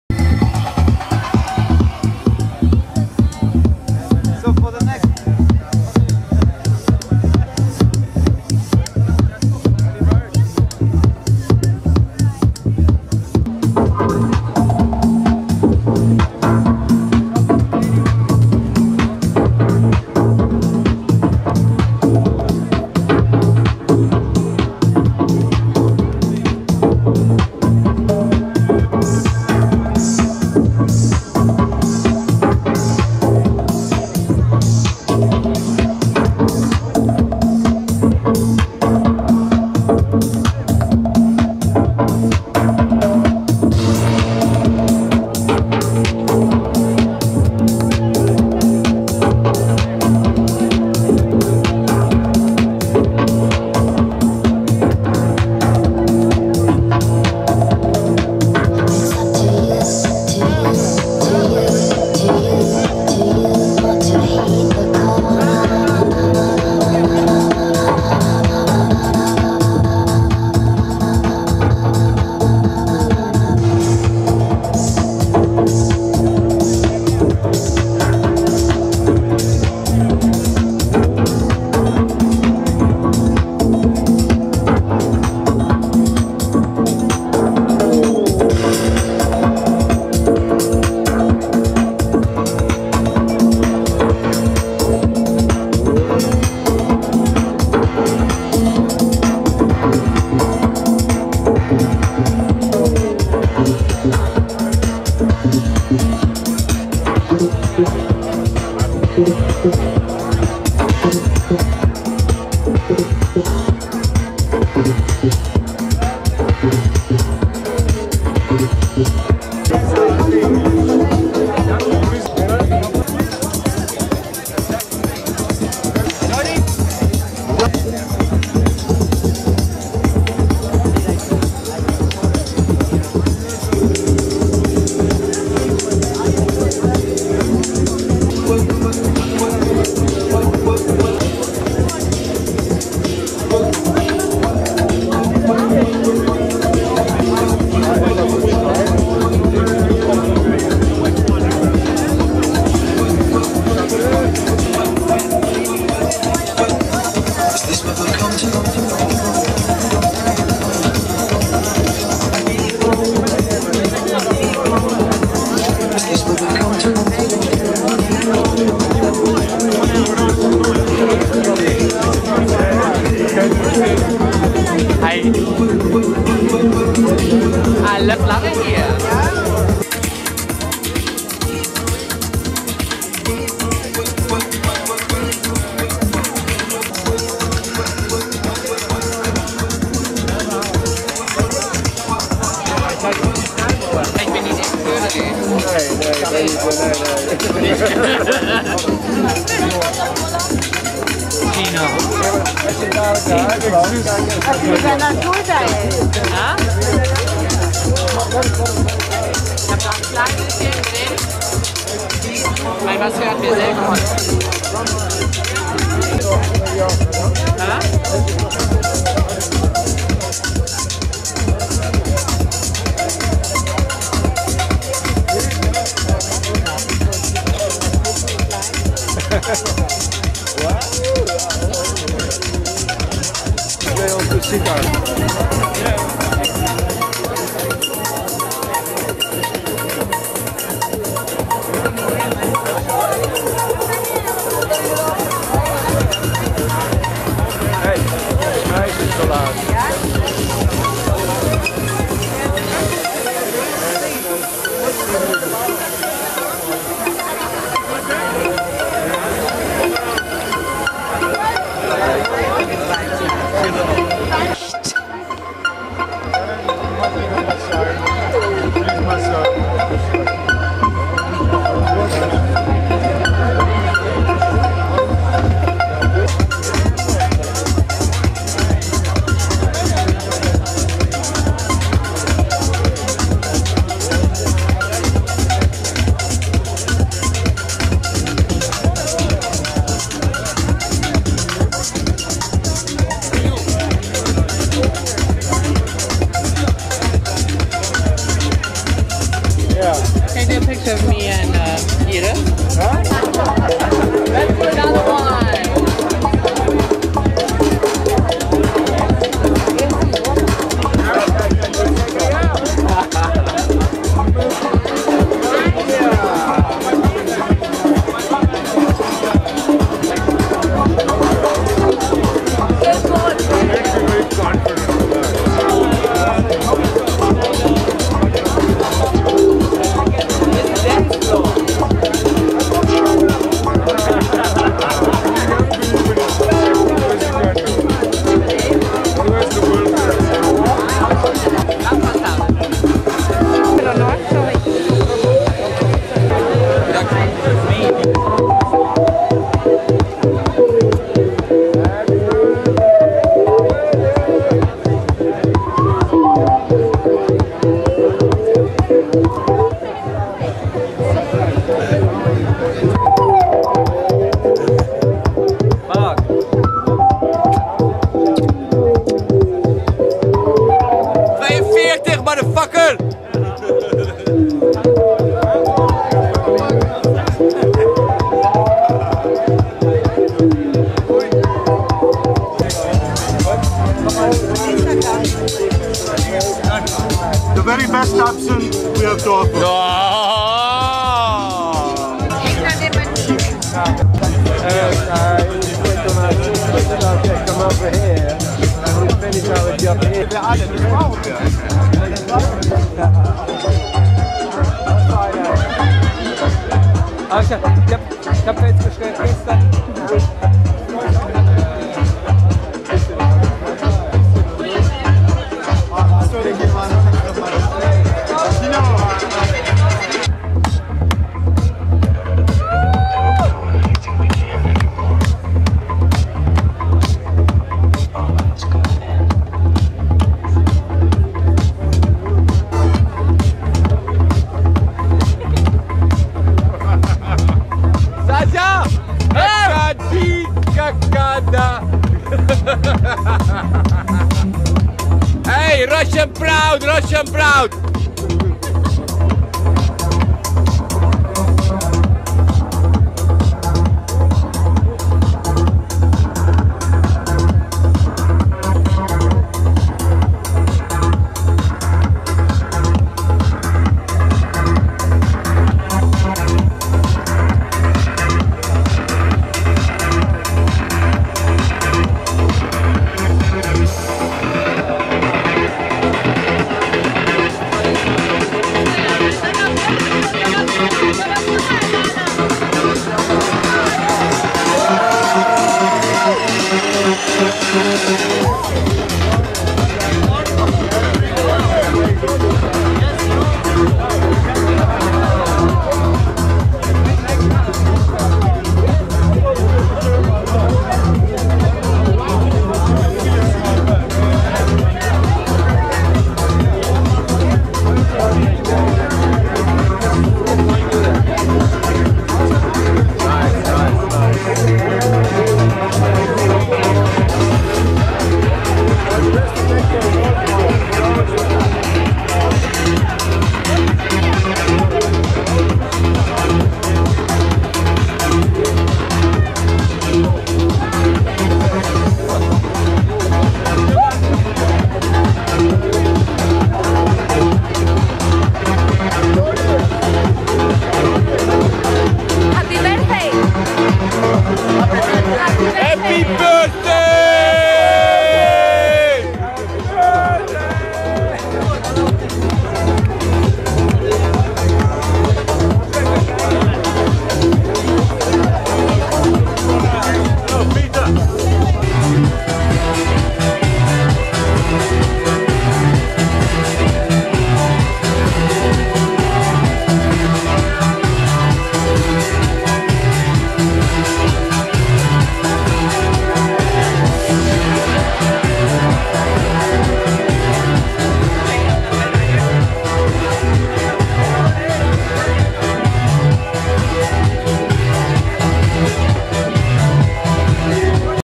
INGO'S SATURDAY NIGHTMARKET dj dhr robijn birthday party
dhr robijn had his b-day in de popular cocktailbar @ the famous ingo's saterdaynightmarket in goa.